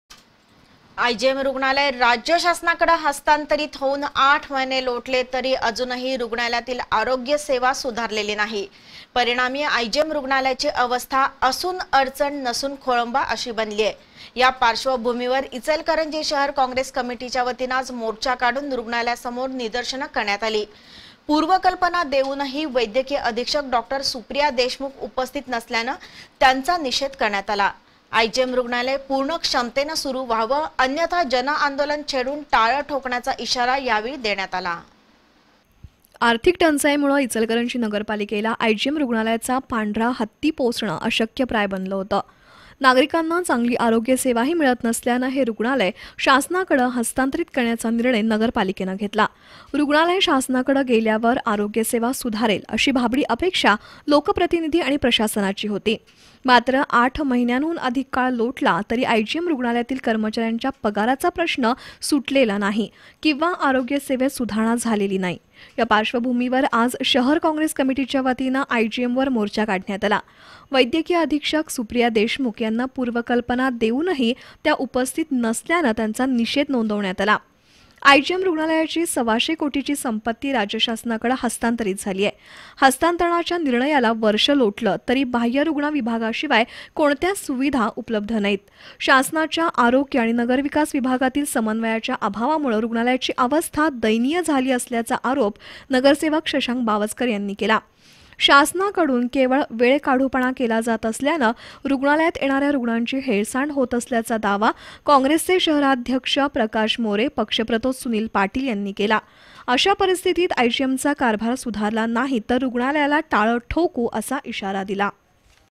इचलकरंजीतील आरोग्यसेवेचा बोजवारा, कॉंग्रेसचा मोर्चा आणि निदर्शनं
इचलकरंजीतील आयजीएम रुग्णालय शासनाकडं हस्तांतरीत होऊन आठ महिने उलटले तरी आरोग्यसेवेचा बोजवारा, कॉंग्रेसचा मोर्चा आणि निदर्शनं